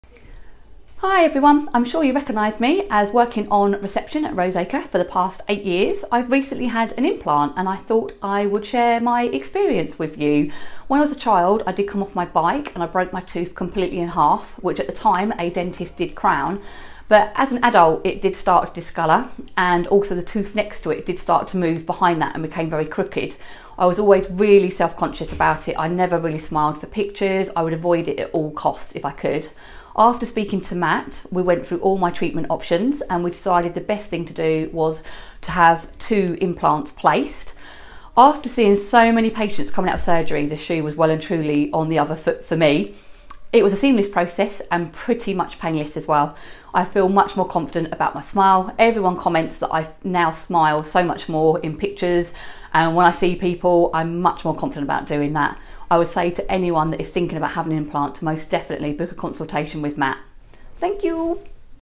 Renee’s Dental Implant Journey
Renee describes the confidence that her new dental implants have given her.